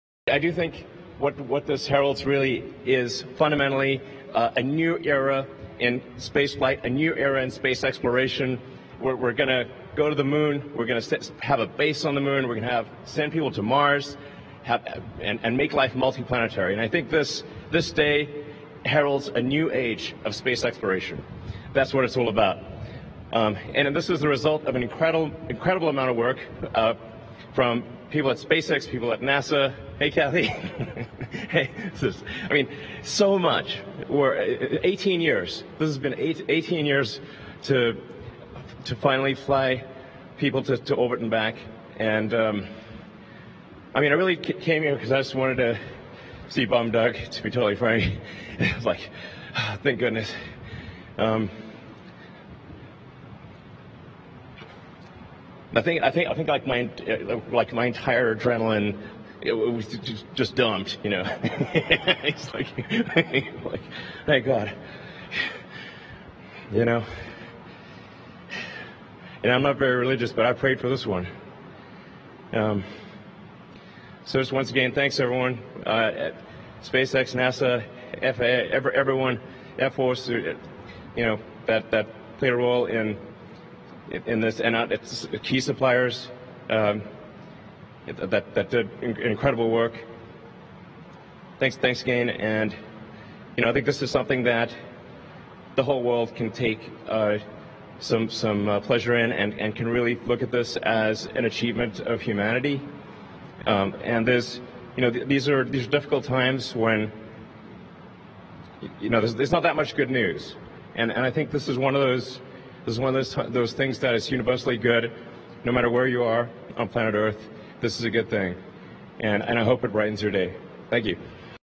Elon Musk | On Crack!
Starship will take humanity to the Moon in less than 5 years, and Mars will be 7-8 years.

"We gonna go to the Moon, We gonna have a Base on the Moon, We gonna send People to Mars and make Life Multiplanetary"

-Elon Musk